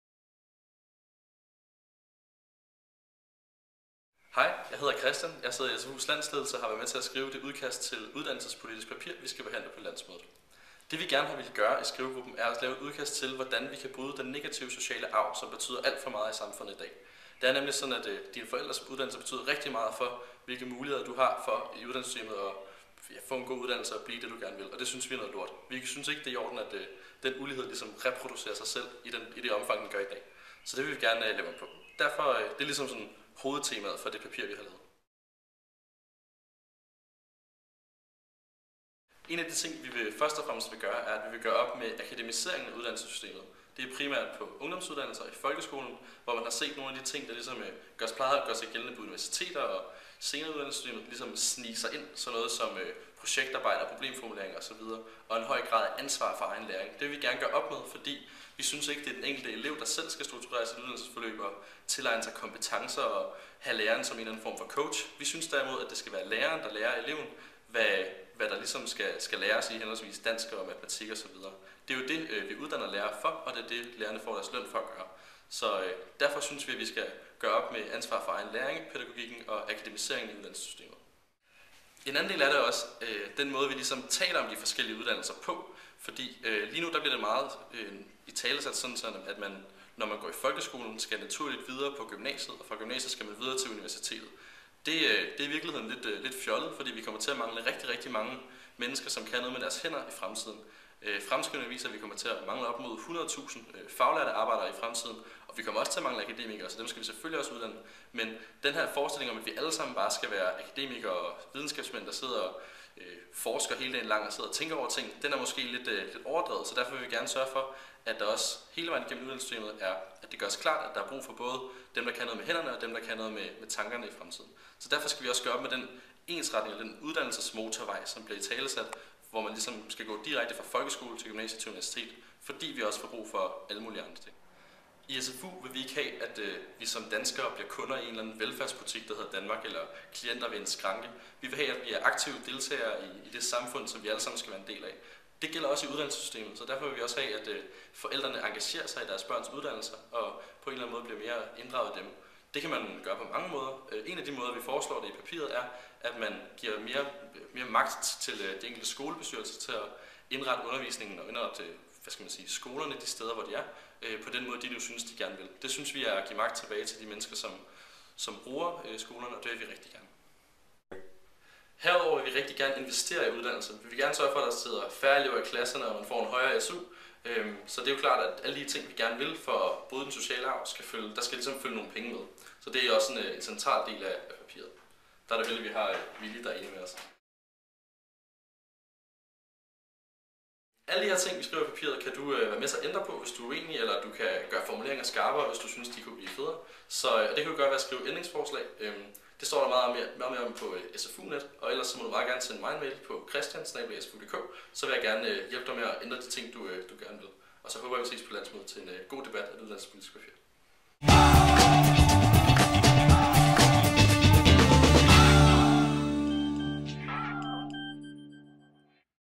Om det uddannelsespolitiske papir til behandling på landsmødet 2011
På SF Ungdoms landsmøde 2011 skal vi blandt andet behandle et papir om uddannelsespolitik. Læs mere på